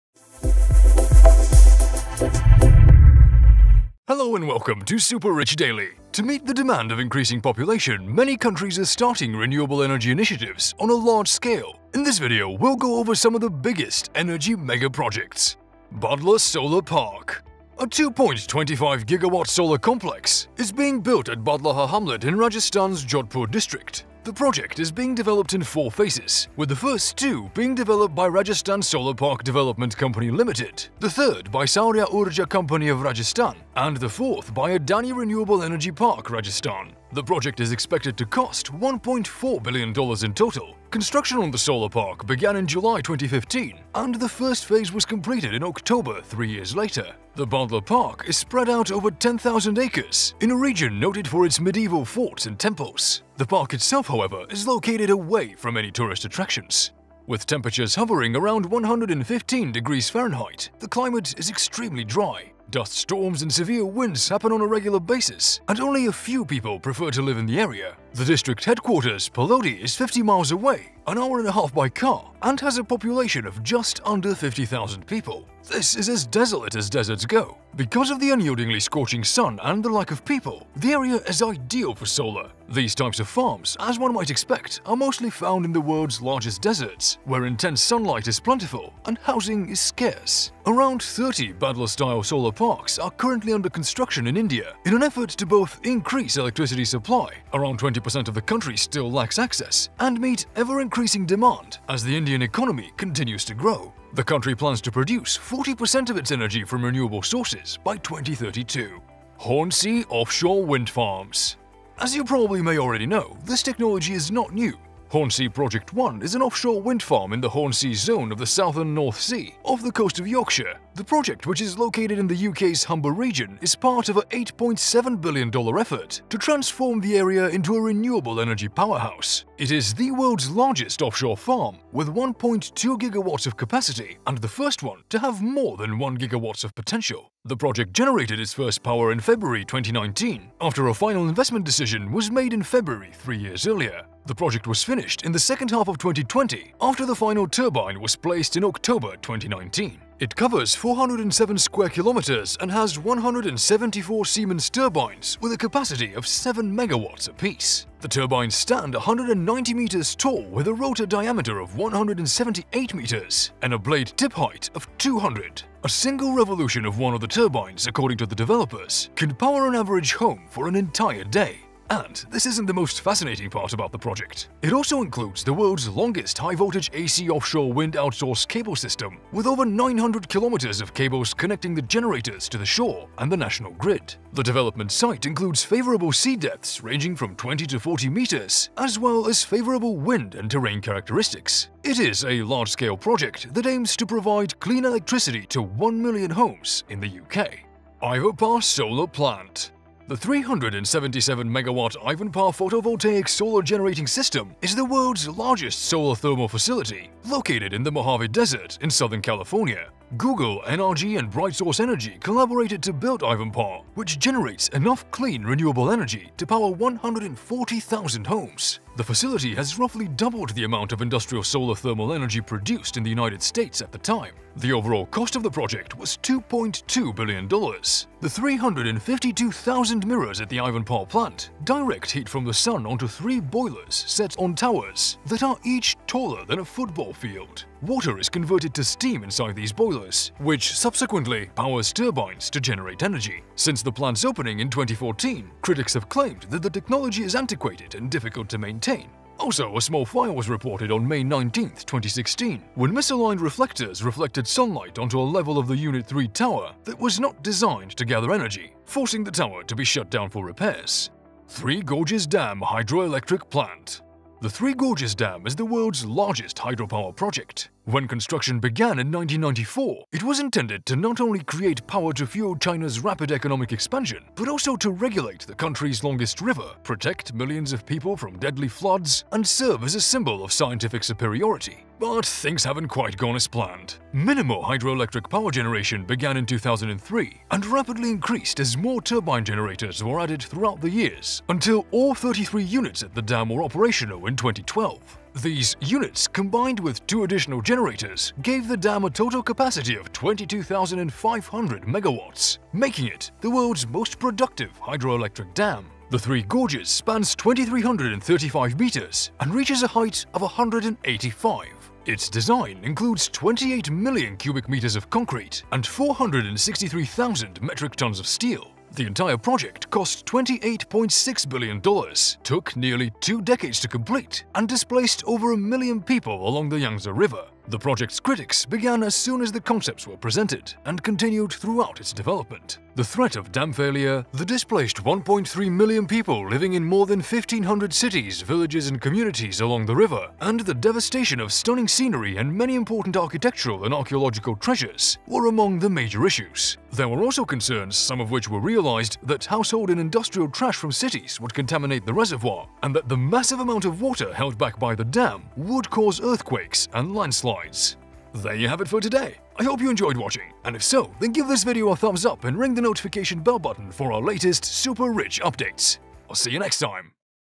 World's Biggest Planned Renewable Energy Mega Projects | Upcoming Major Energy Projects
Renewable energy is the most crucial  sector in the World right now hence the fastest growing sector globally, with more renewable energy projects receiving funding than ever before. As the need for clean, sustainable energy increases, and renewable technologies get ever more advanced, more and more projects are being developed in greater sizes and complexities.

In this video we are going to have a look at some of the biggest energy projects in process all around the World. these renewable energy mega projects are planned to meet energy requirements of big cities and play a crucial role in replacing the traditional fossil fuels.  Here are some of the world’s largest proposed renewable energy projects which demonstrate that the energy transition is well underway and renewable energy is nothing but the future.

Sven